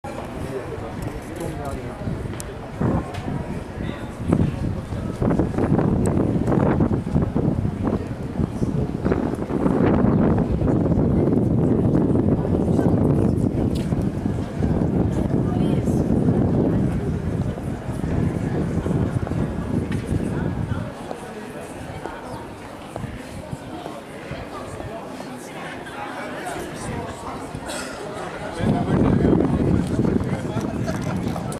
Privilodges Le Royal - Annecy
A l'extérieur du Privilodges Le Royal*** à Annecy , vous trouverez des endroits pour se balader et des bars-restaurants. C'est un endroit plein de convivialité !